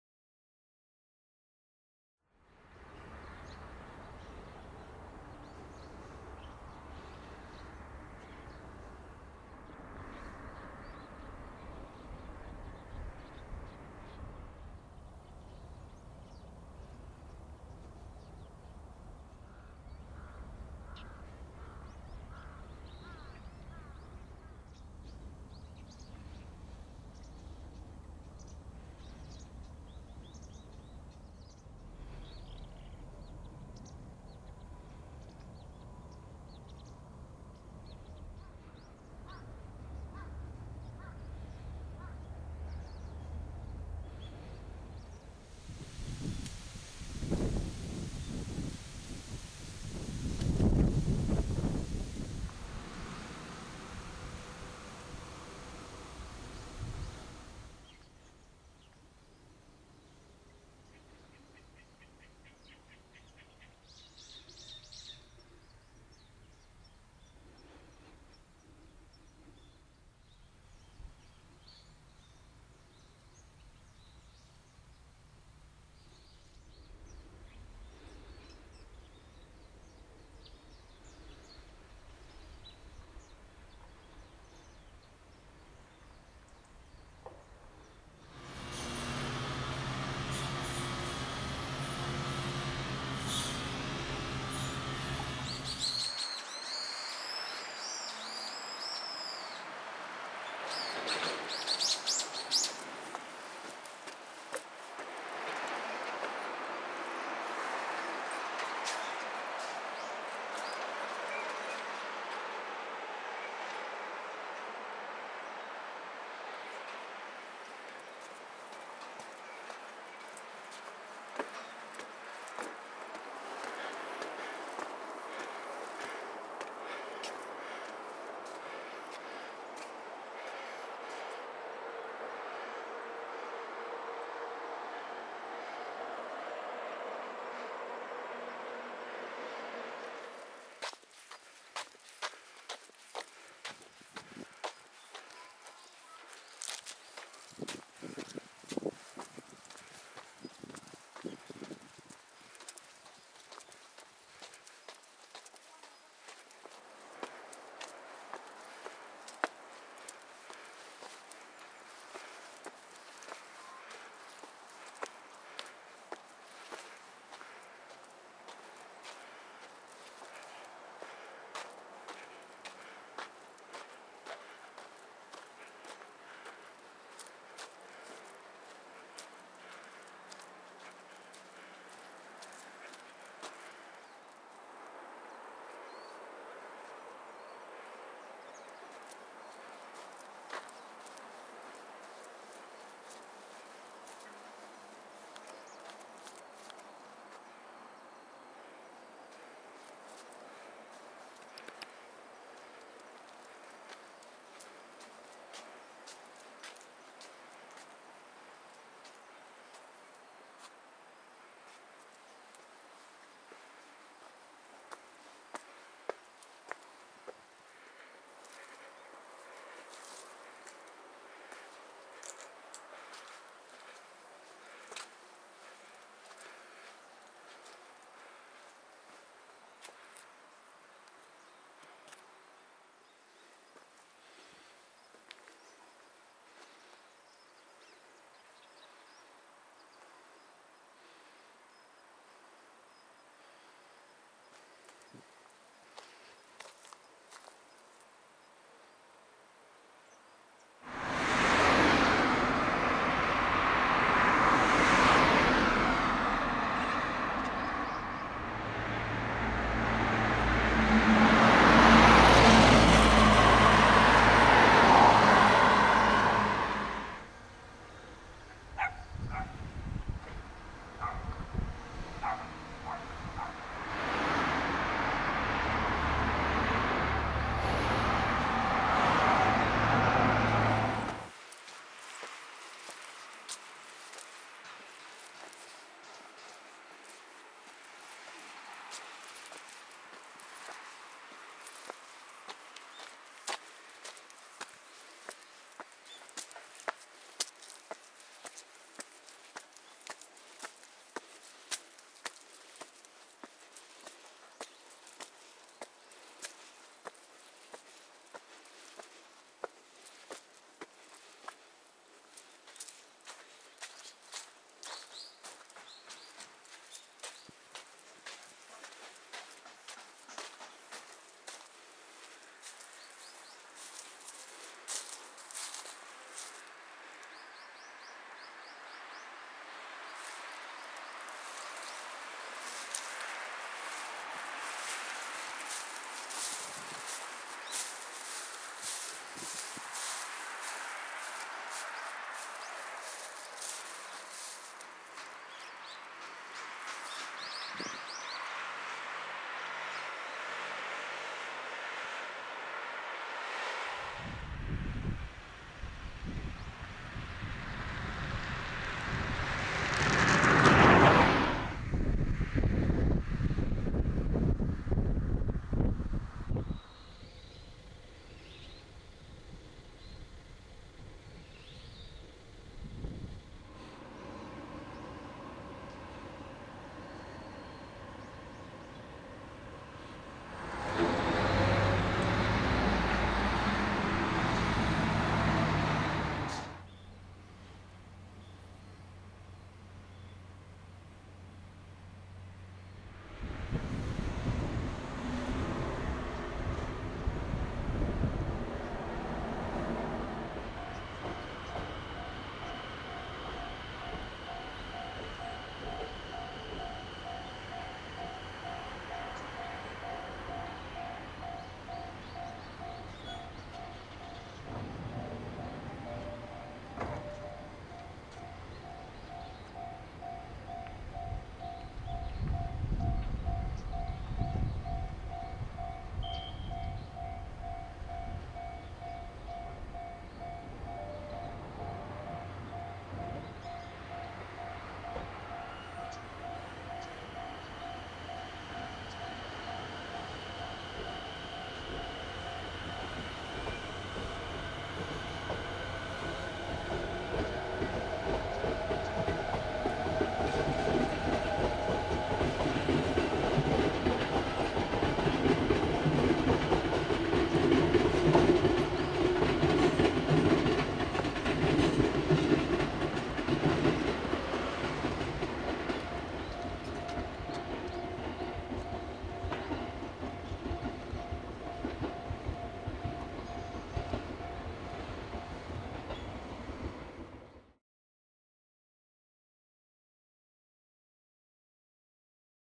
ぐるっと香取市（小見川あたりをブラブラと）
11/16（月）JR水郷駅が旧小見川町の北限だね。一ノ分目
黒部川沿いから阿玉台貝塚～城山公園～大塚山古墳～水郷液まで。